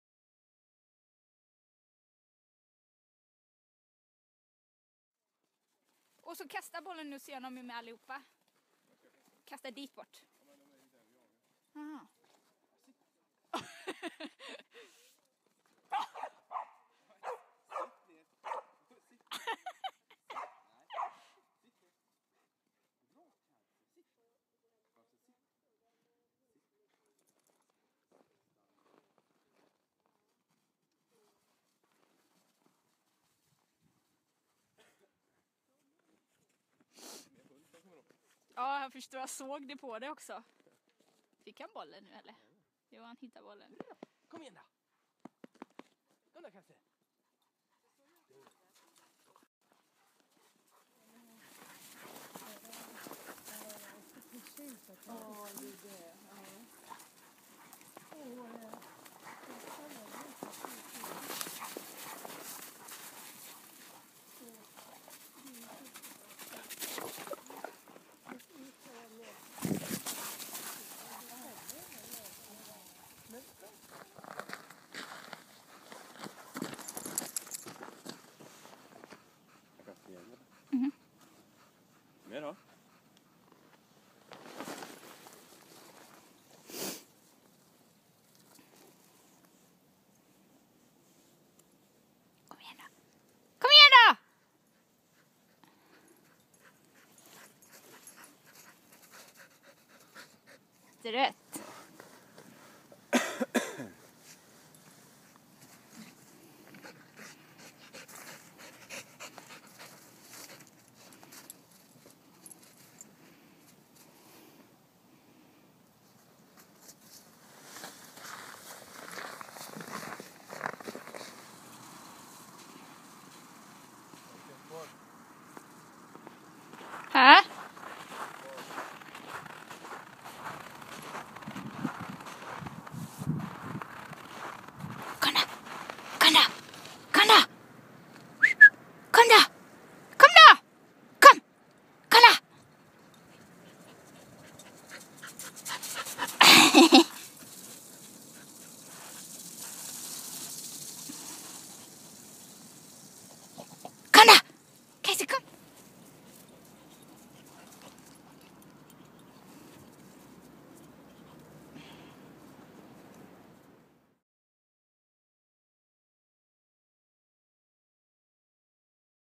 A Day In the Snow
The Samoyed Kaiser Is Playing With His Buddie's Bamse